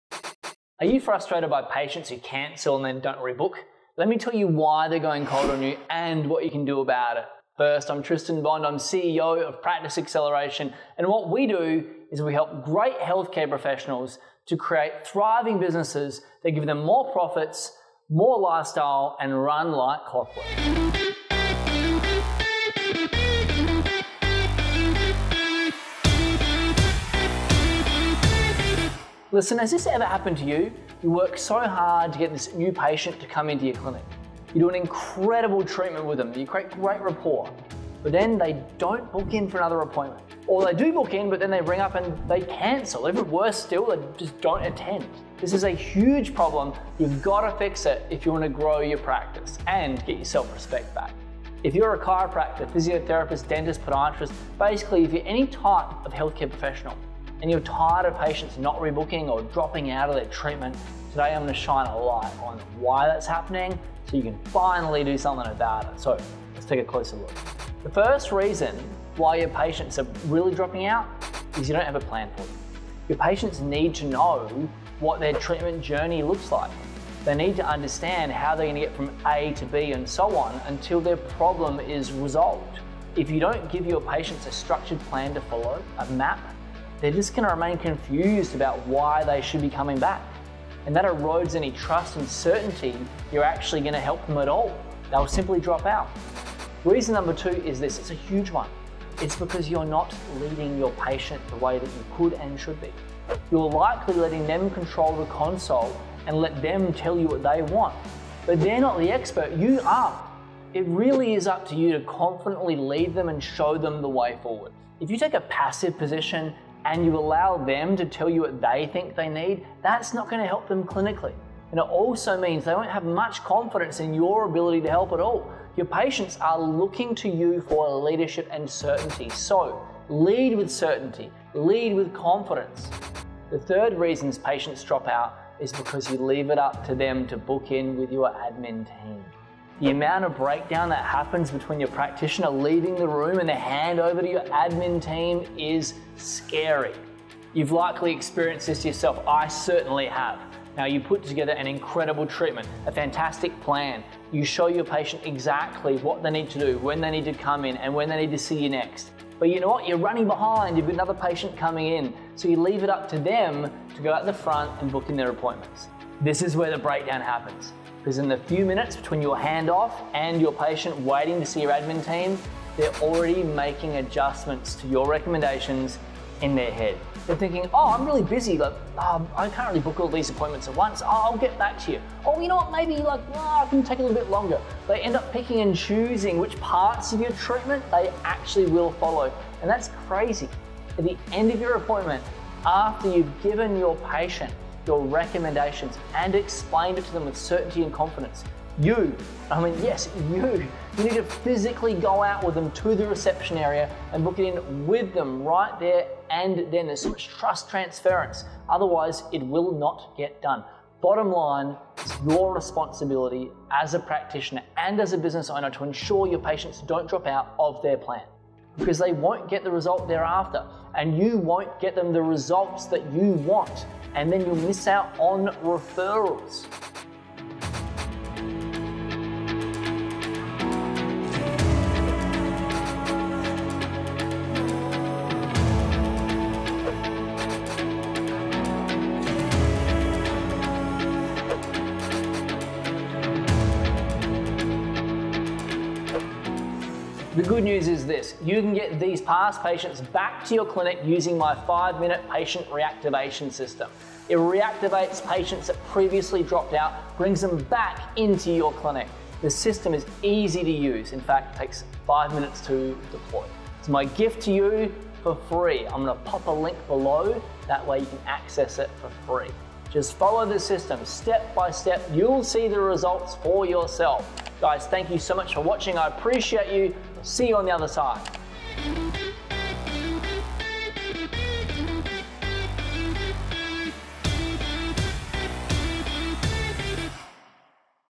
Patient Drop Out? Here's 3 Reasons Why They Don't Rebook
Learn more about patient retention today!

As someone who runs a physiotherapy clinic, a dental practice owner, or healthcare practitioner running your business, you're probably frustrated by patients who cancel and then don’t rebook appointments. Today, I talk about 3 reasons why they might be doing this, and how you can improve your patient bookings for a more profitable practice.

Getting new patients isn't always easy, and when you do get them, you work hard to give them good service so you can gain more patient bookings from them, and for overall patient retention. But let's face it, some of them don't book in for another appointment, drop out of their treatments, or book, cancel, and then never show up again.

As a dental practice owner, physiotherapy clinic owner, or healthcare practitioner, you've GOT to fix this problem in patient retention if you want to grow your practice and have a continuous stream of patient bookings.

So, why are your patients dropping out?

First, it might be because you don't have a plan for them. Your patients need to know what their treatment journey looks like for them to stay with you. How will your treatments resolve their problem? They need to know why they need to come back so you can set more patient bookings with them.

I share the 2 other reasons why patients are likely dropping out, and what you can do as a dental practice owner, physiotherapy clinic owner, or healthcare practitioner to improve patient retention. Keep watching this video to know more.